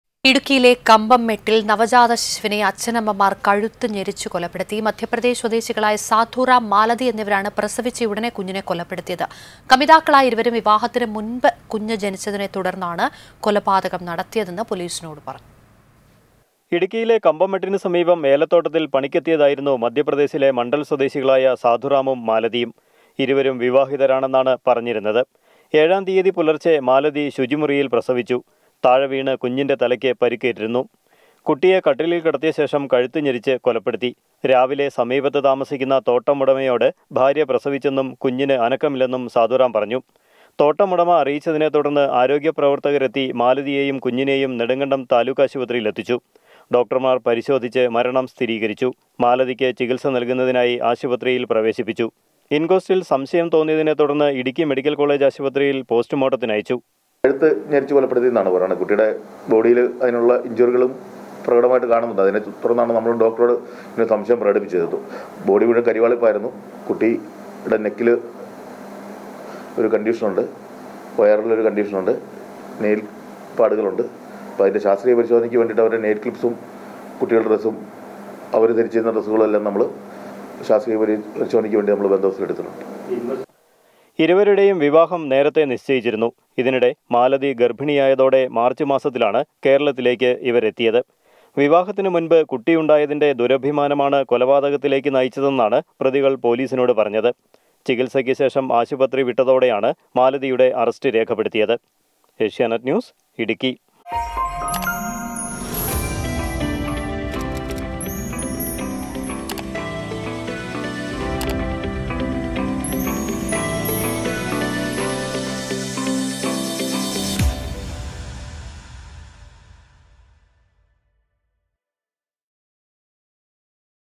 നവജാത ശിശുവിനെ മാതാപിതാക്കൾ കഴുത്തു ഞെരിച്ച് കൊന്നു| Infant| Idukki
ഇടുക്കി കമ്പംമേട്ടിൽ പ്രസവിച്ചയുടനെ കുഞ്ഞിനെ മാതാപിതാക്കൾ കഴുത്തുഞെരിച്ച് കൊന്നു, വിവാഹത്തിന് മുമ്പ് കുഞ്ഞ് ജനിച്ചതിനാൽ കൊലപ്പെടുത്തിയെന്ന് പൊലീസ്; കൊലപ്പെടുത്തിയത് മധ്യപ്രദേശ് സ്വദേശികൾ
Infant| Idukki